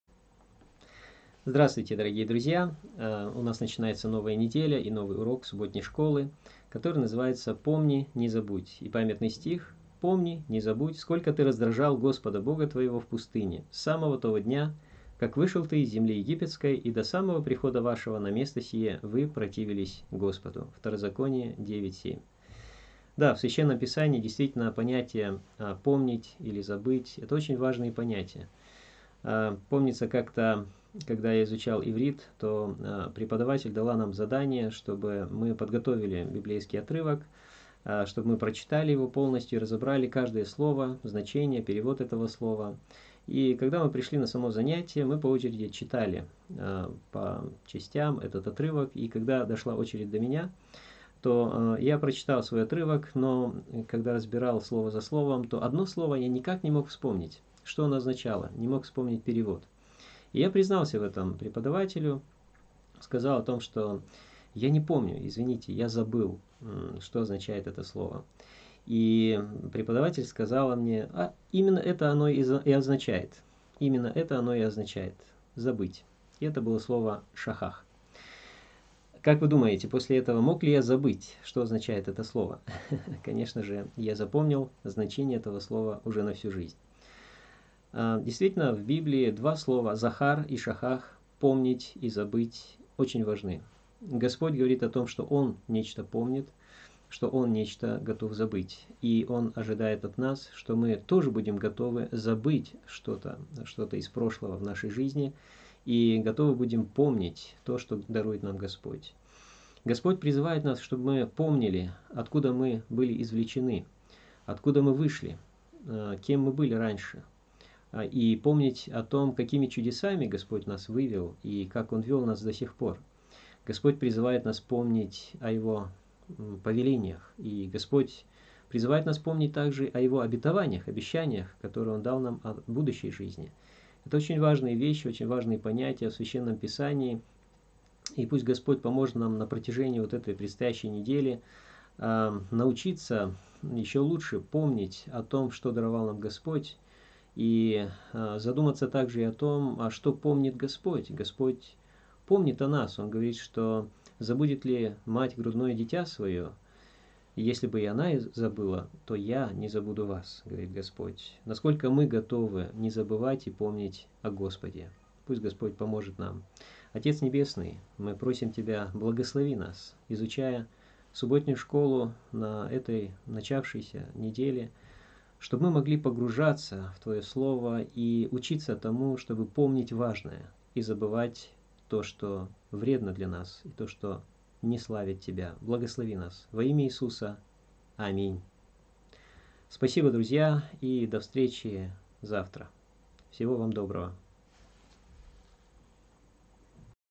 27 ноя 2021. Помни, не забудь. Введение
Что нам следует помнить? О чем стоит забыть?

Местная религиозная организация Церковь Христиан Адвентистов Седьмого Дня г. Нальчика.